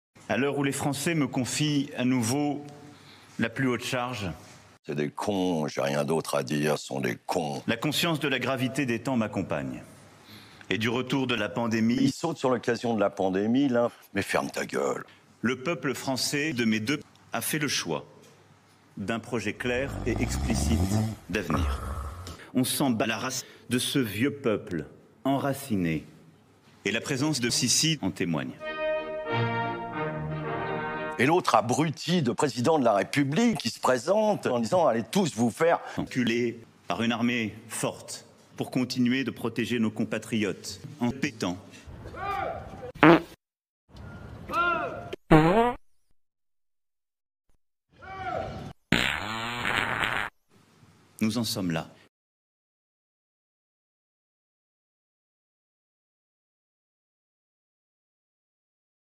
MACRON : "On s'en bat la race..." feat. François Cluzet
Emmanuel Macron est à la recherche de son nouveau premier ministre. Et si la surprise était de taille. Le Président est-il sur le point de nommer François Cluzet à la tête du gouvernement ? Tortech Prod mène l'enquête !